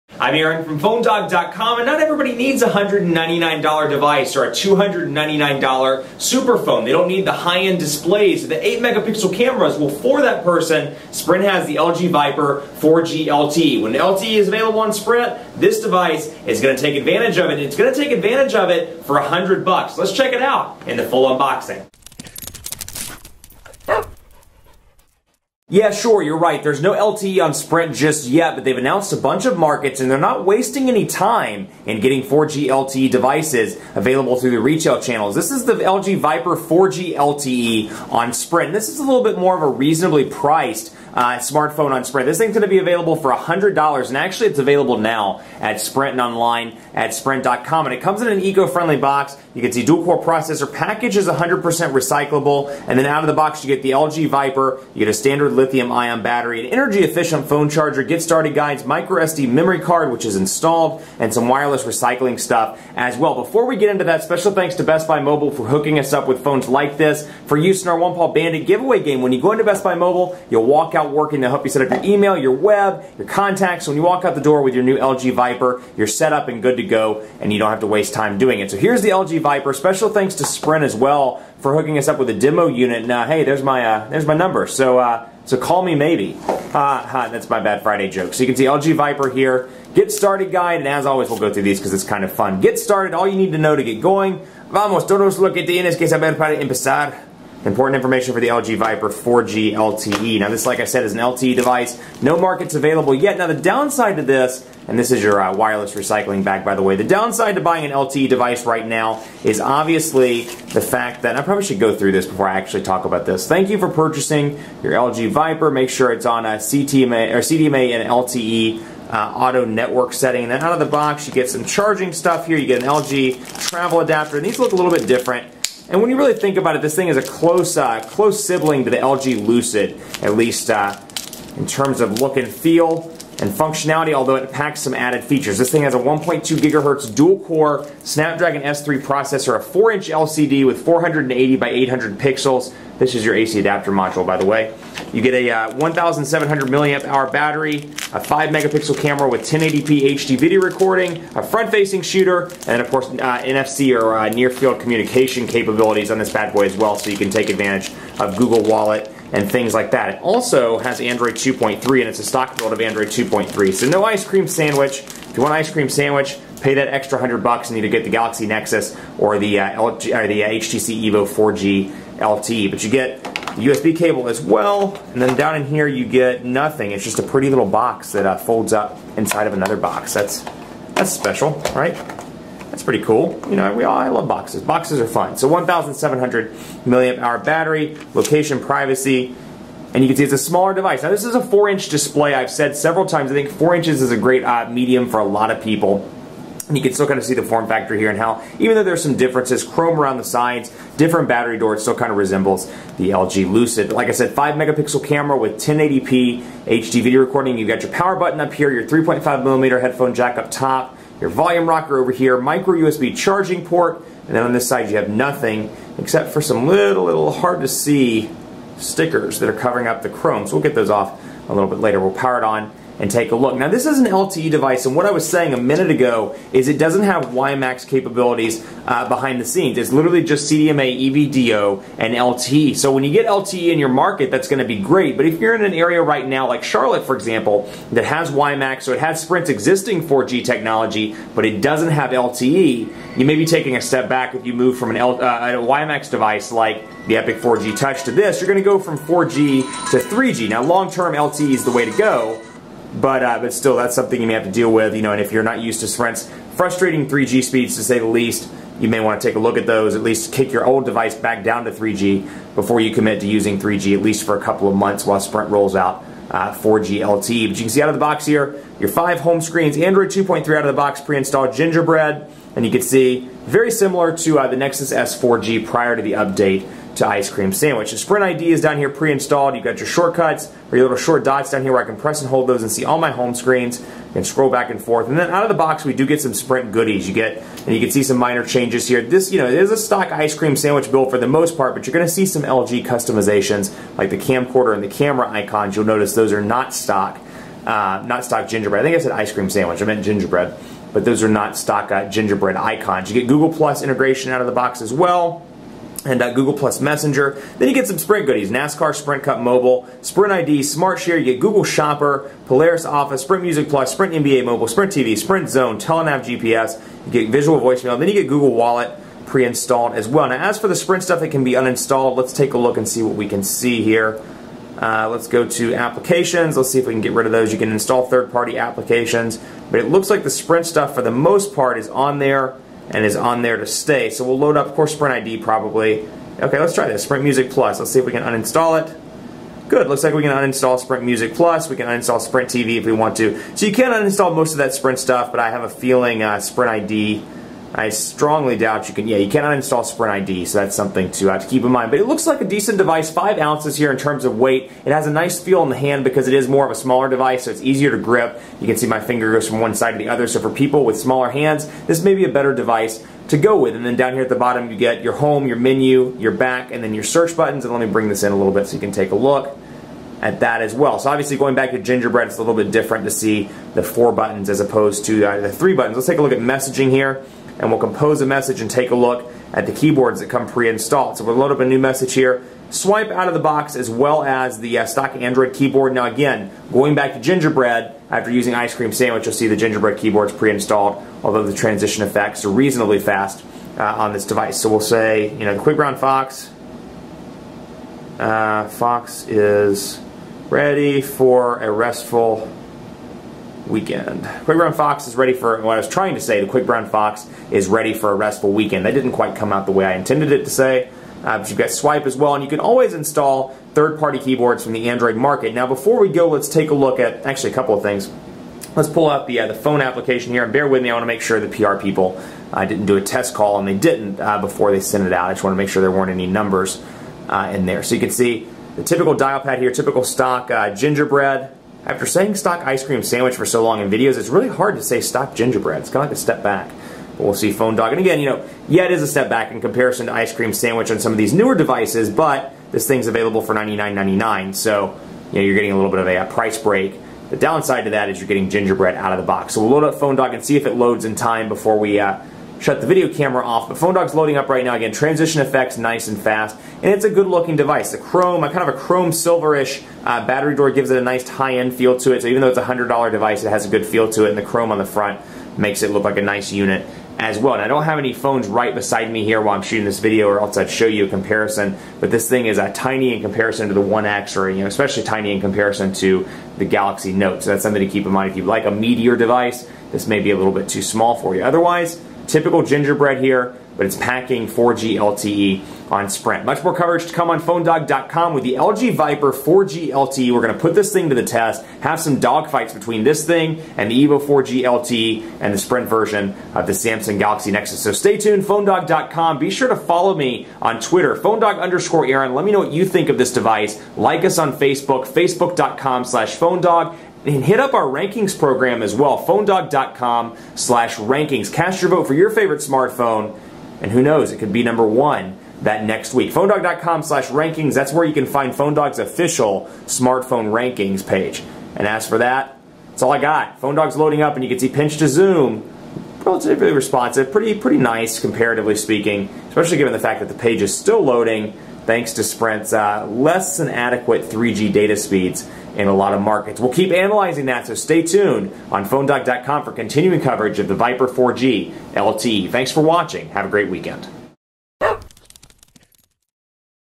LG Viper 4G LTE Unboxing
Aaron unboxes the LG Viper 4G LTE, one of Sprint's new 4G LTE handsets.  The Viper offers a 1.2 GHz dual-core Snapdragon S3 processor, 4-inch LCD with 480x800 pixels, 5-megapixel camera with 1080p HD video recording, a front-facing camera, 1,700 mAh battery, and stock Android 2.3 (Gingerbread).  It's available now for $99.99 at Sprint.  Is it worth the Benjamin, or should you go with something else?